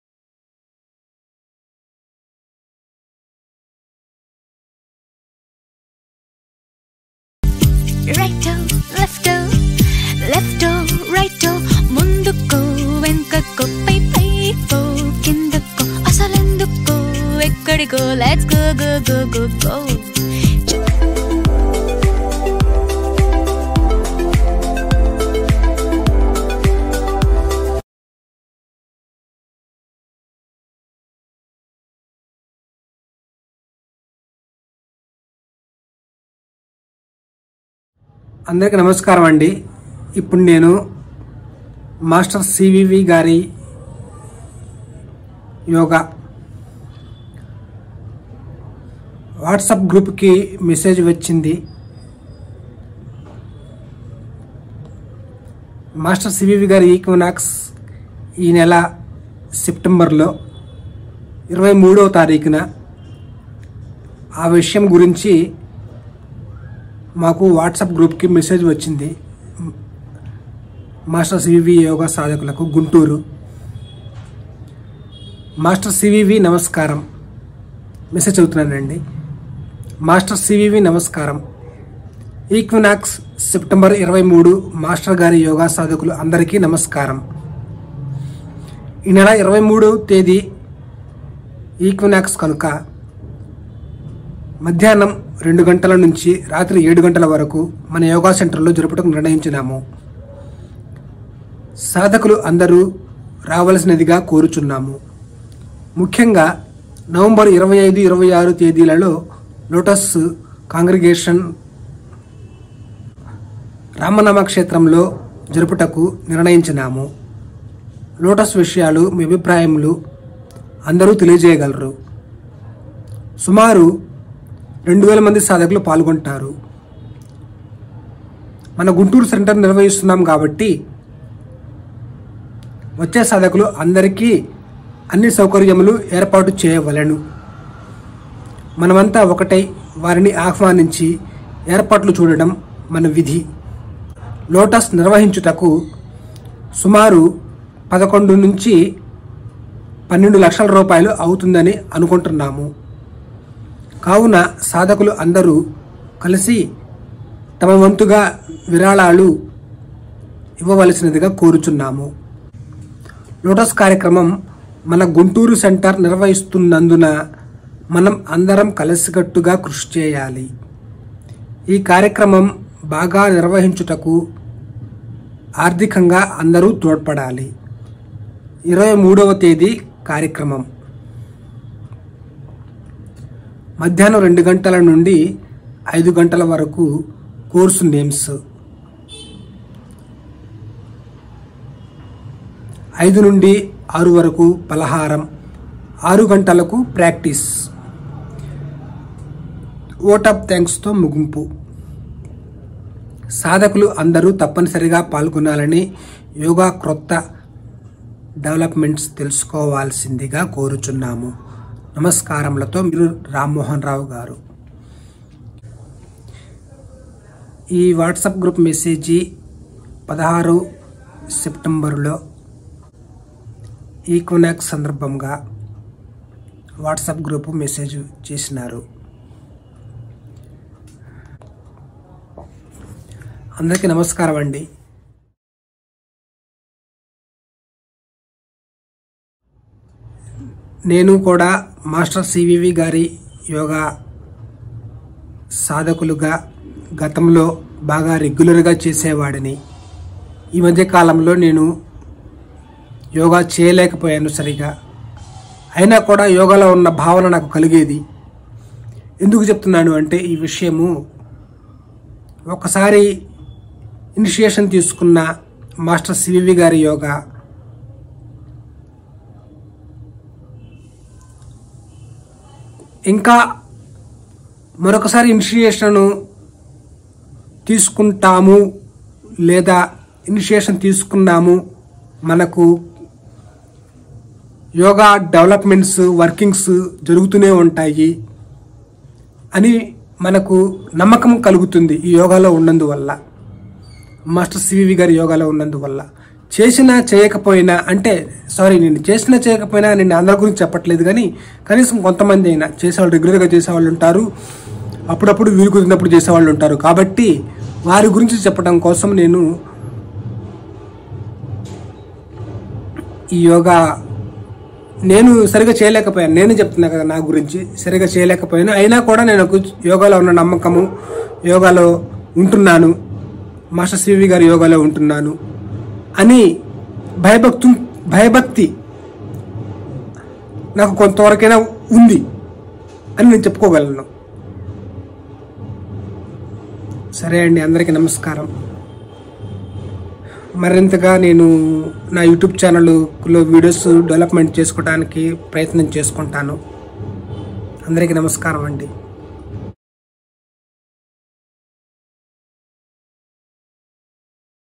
Master C.V.V yoga equinox and 22nd LOTUS congregation | WhatsApp group message | [ 4K ]
Announcement of master C.V.V yoga equinox
September 2023
Saturday 23
Guntur Yoga center